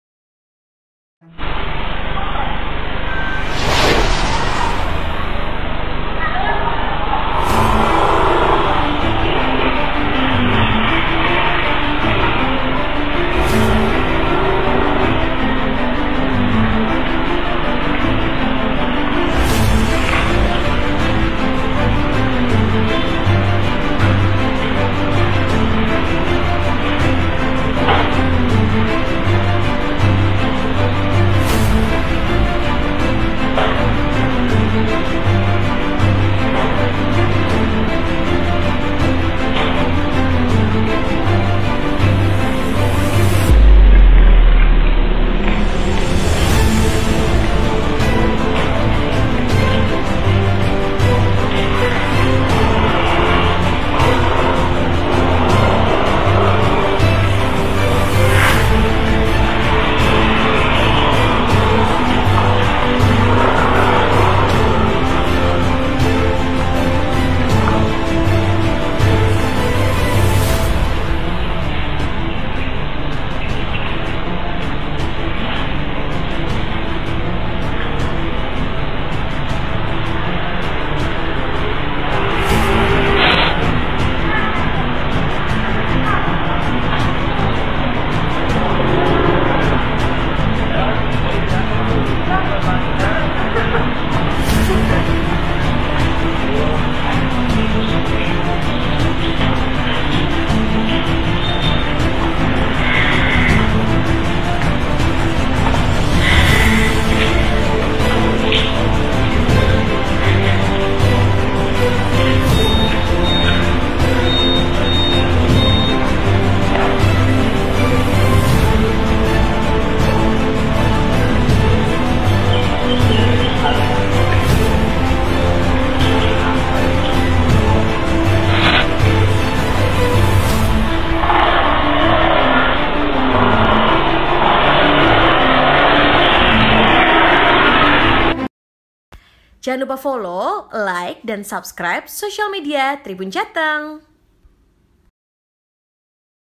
Detik-detik Pria Macak Alim Curi Motor di Masjid
Detik-detik seorang pria berpeci dan bersarung curi motor Honda Scoopy berwarna merah dari halaman sisi selatan masjid.

Tampilan wajah pria itu terlihat jelas karena ia sempat menoleh ke arah bawah CCTV.

Langkahnya menjauh dari CCTV, menyusuri area wudhu dan mendekati tiga sepeda motor yang berjejer menghadap ke arah barat.

Wajah pria itu juga terlihat ketika melintas di bawah CCTV saat memacu motor tersebut ke luar area masjid.

Editor : Yga